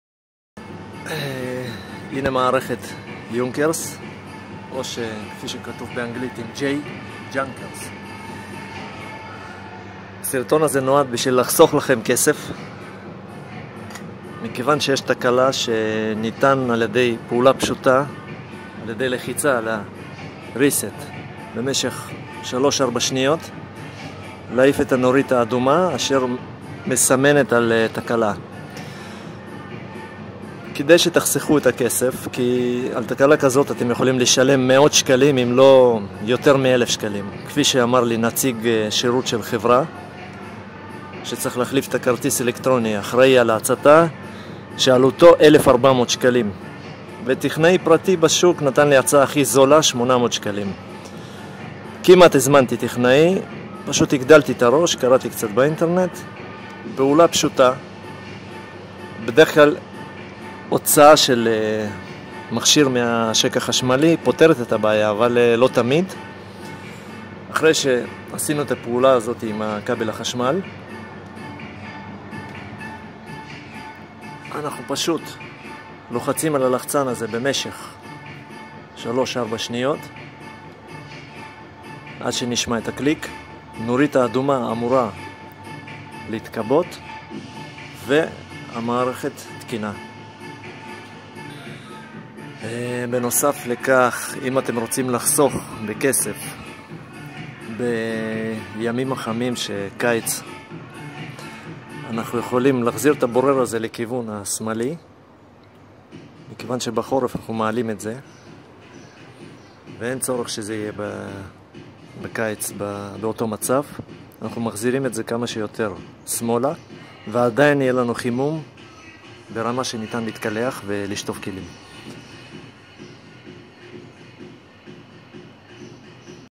מחמם מים על גז יונקרס junkers
תקלה  מחמם על גז לא מחמם אין ניצוץ דורש איפוס